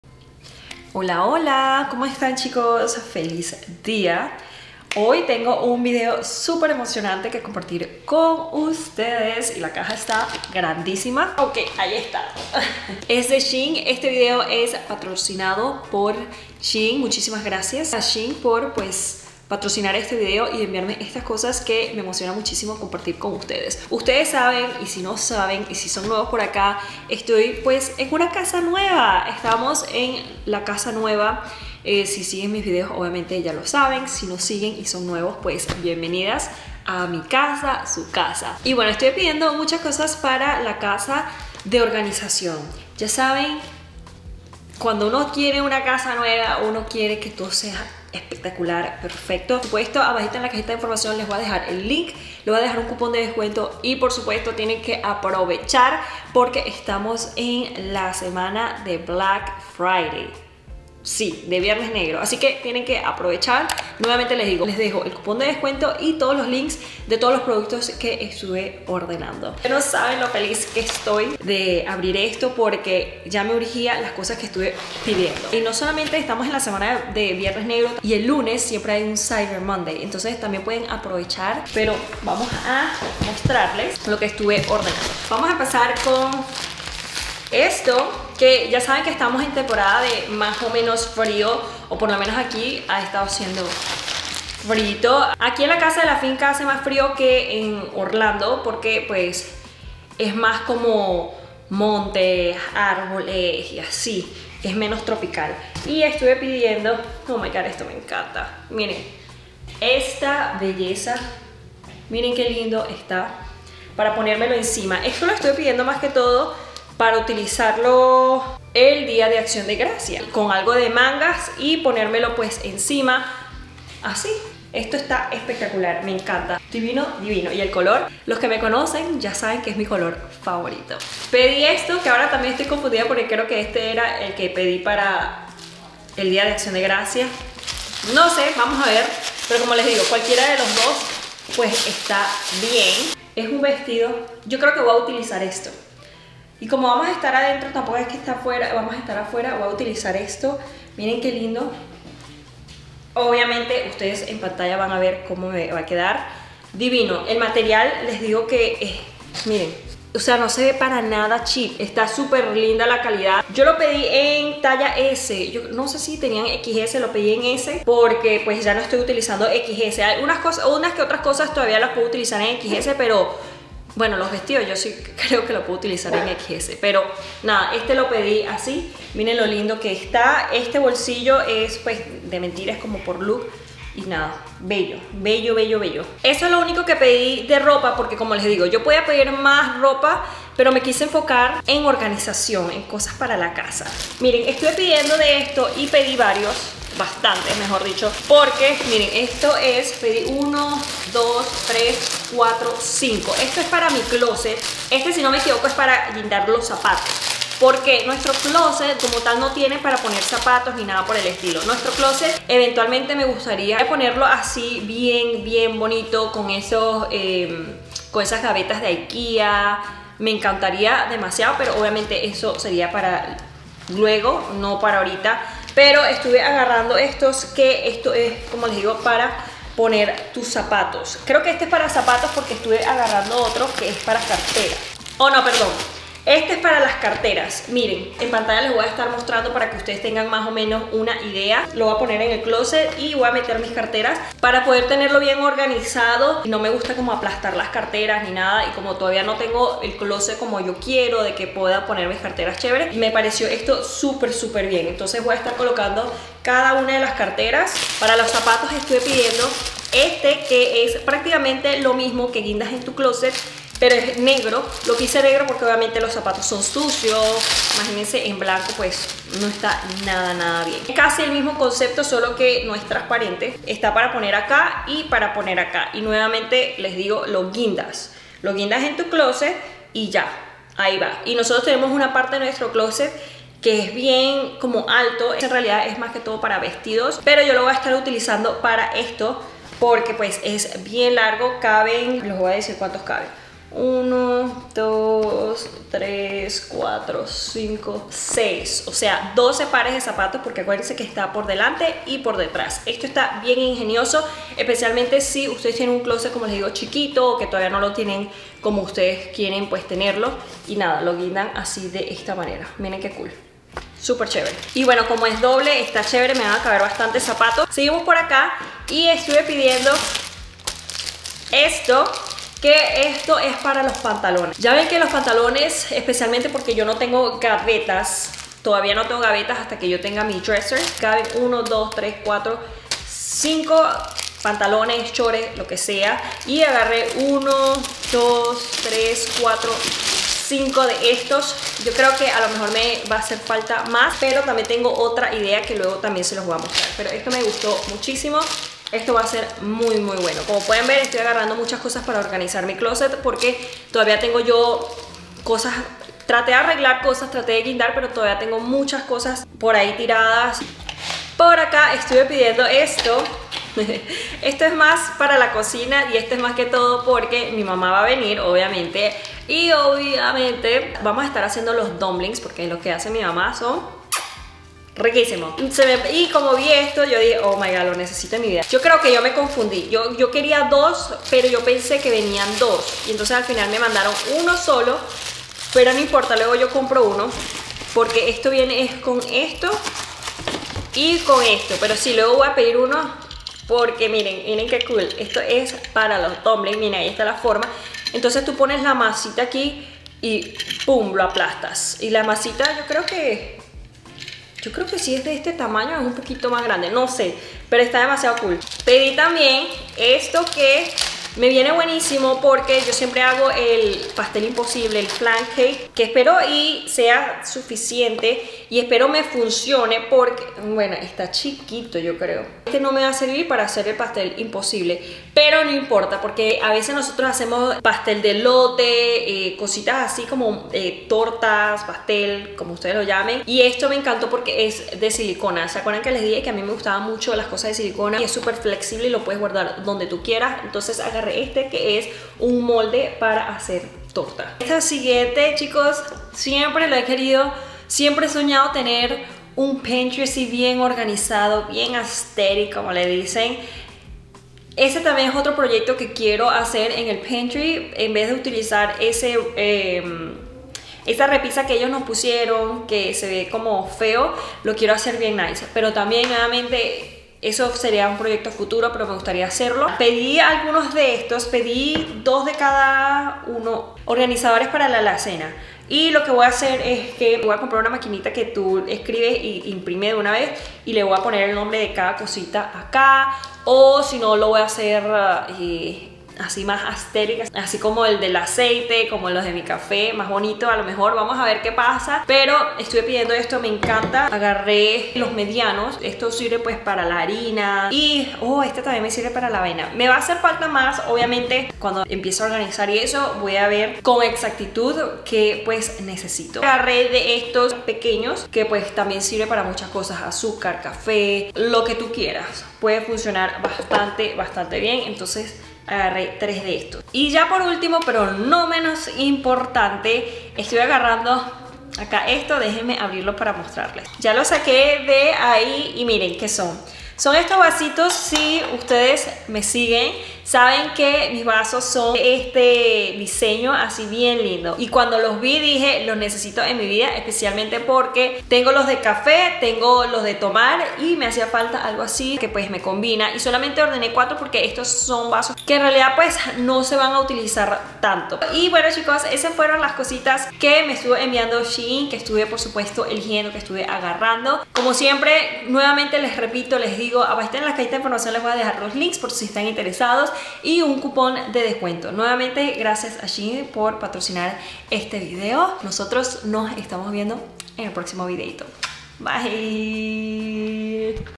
Compras De SHEIN Para La Organización De La Casa Nueva.. Vale La Pena Comprar!!
2pcs Stainless Steel Dumpling Mold & Cutter Set,Small Stainless Steel Dumpling Wonton Mould, Easy-tool For Dumpling Wrapper Dough Stamp Cutter Pastry Pie Making 9719834
SHEIN Basic living 1Pc Clear Food Storage Canister with Bamboo Lid, for Flour Cereal Sugar Tea Coffee Beans Snacks 17292649
SHEIN Basic living 1pc Multifunction Pants Hanger, Grey PP Creative 5 Layer Hanger, For Home Wardrobe 11812611